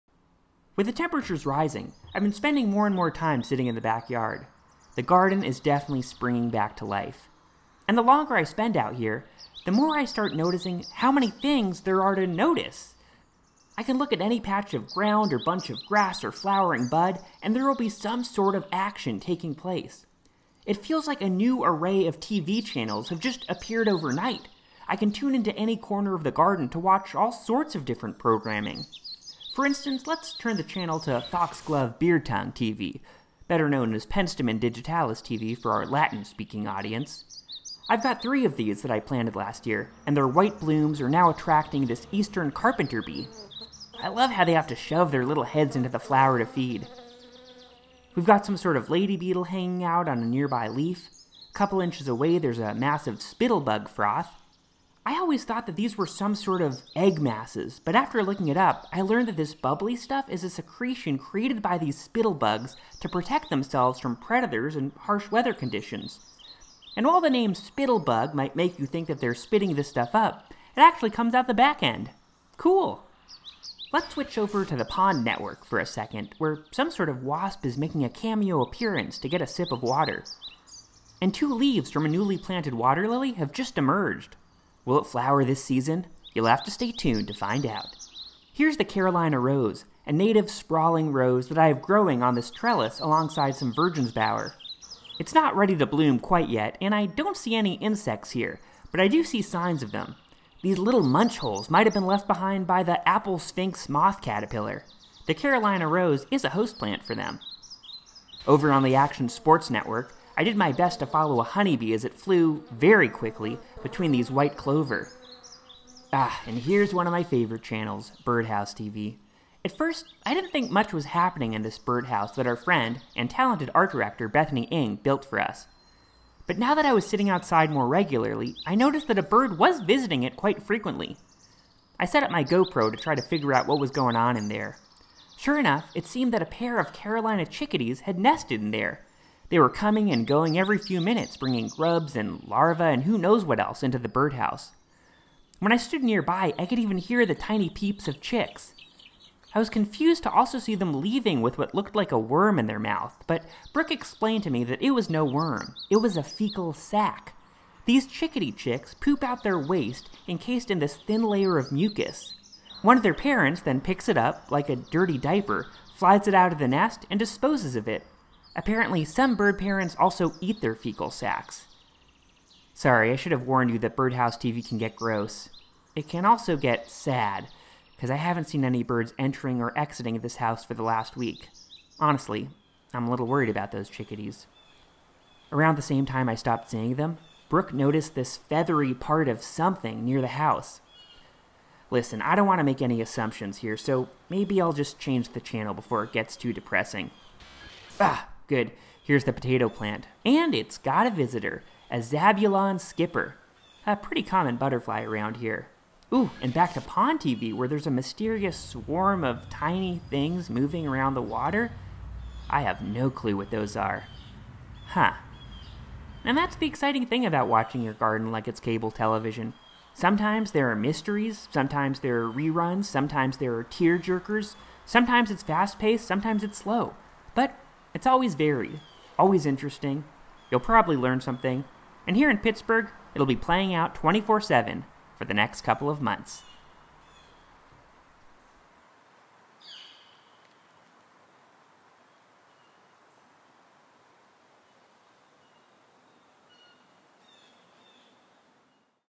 All Bees! All Birds! All the time!
With summer quickly approaching, I've been binging all the wildlife activity in my backyard like it's a new cable channel.